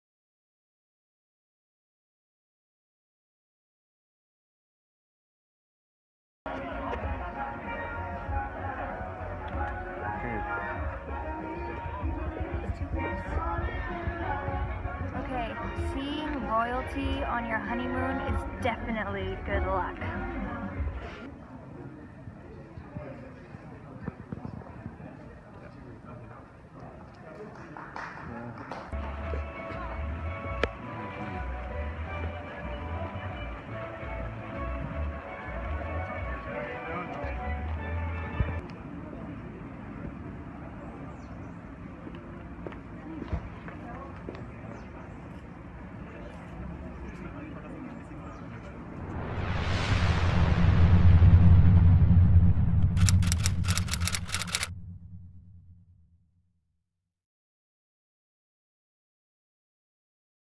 Prince Harry in Nevis!!!
We never expected we'd bump into royalty on our one month long honeymoon in the Caribbean in November 2016. What a treat to have Prince Harry visit while we were staying at the Four Seasons Nevis Resort in St Kitts and Nevis!